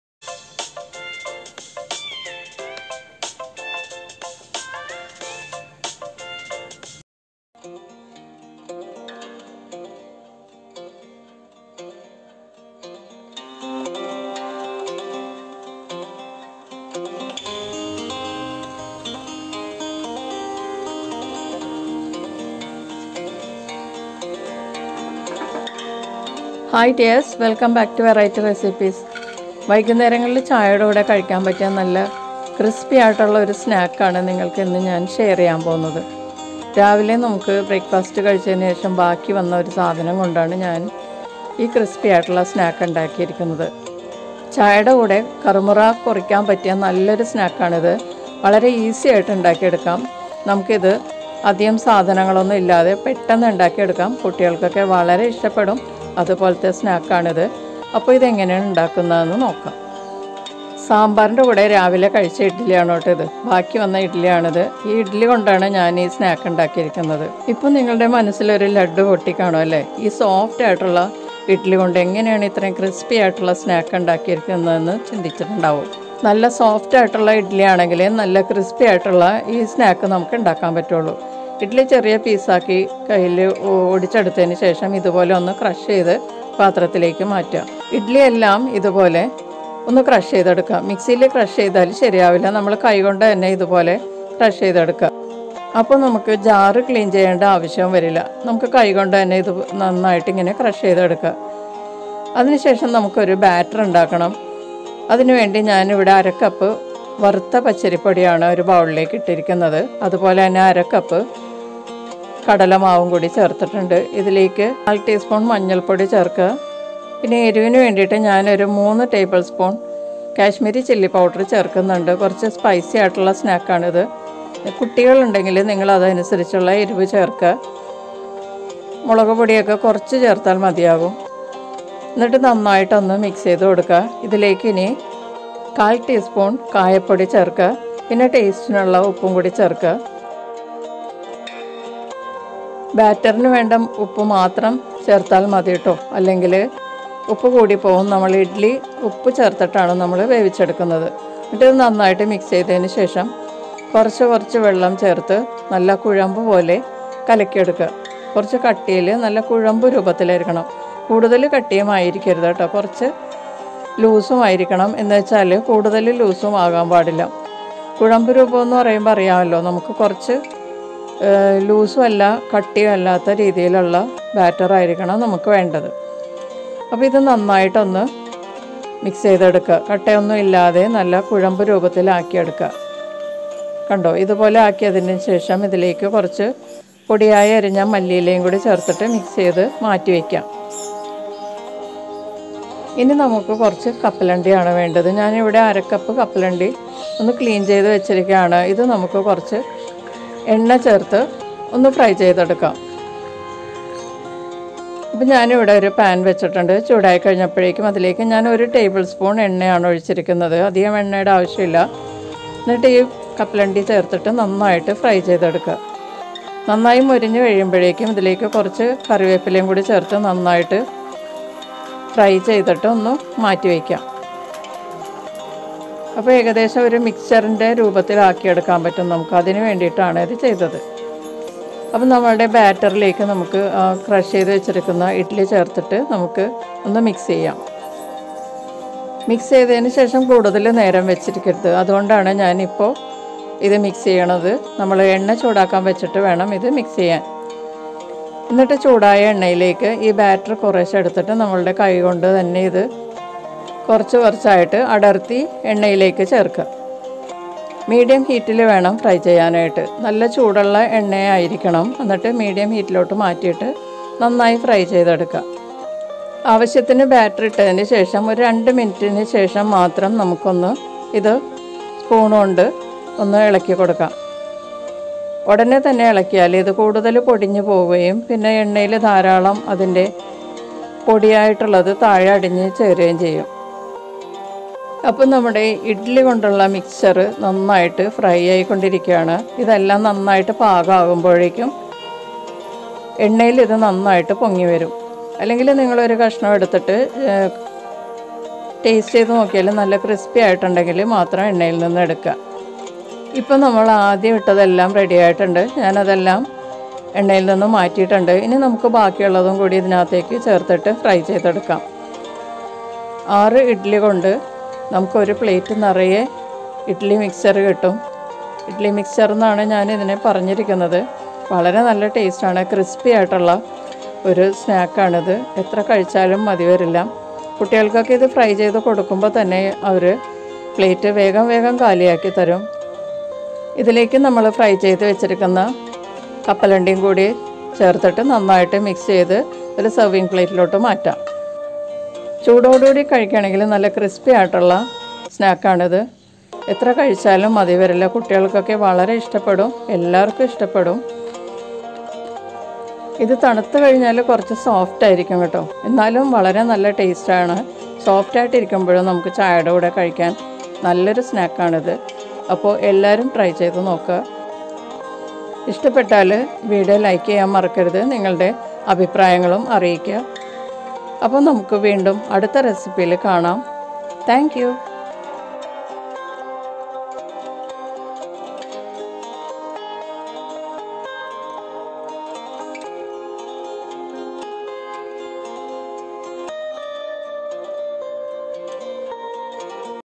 ബാക്കി വന്ന Breakfast കൊണ്ടു കറു മുറ ചായയുടെ കൂടെ കൊറിക്കാൻ Crispy Evening Snack | Variety Recipes
snack.very